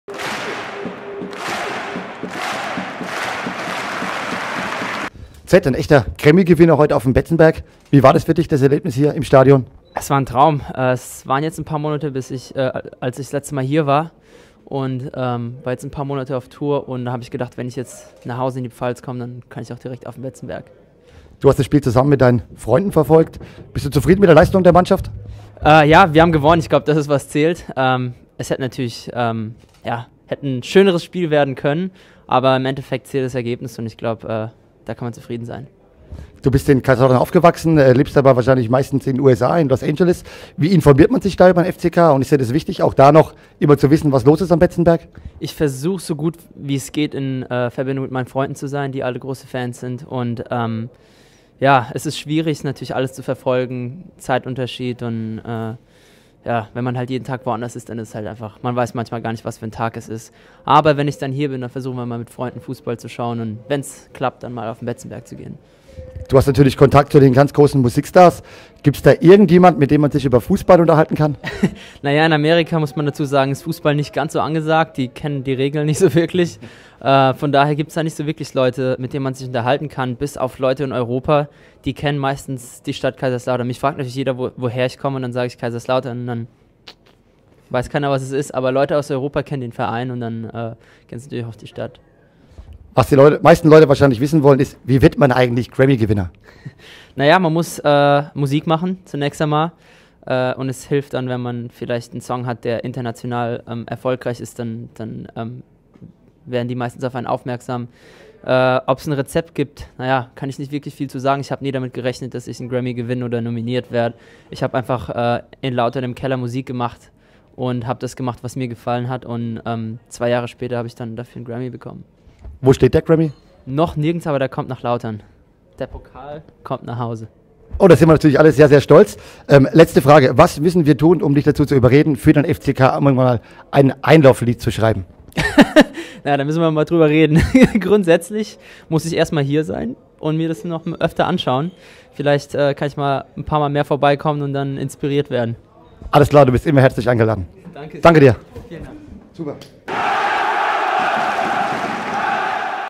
Grammy-Gewinner Zedd besucht den Betze
Ende Januar 2014 wurde Zedd mit dem berühmten Grammy-Award ausgezeichnet. Der junge Musiker, der in Kaiserslautern aufgewachsen ist, besuchte am Samstag, 8. Februar 2014, mal wieder den Betzenberg und konnte einen Sieg der Roten Teufel beim Spitzenspiel gegen die SpVgg Greuther Fürth bejubeln.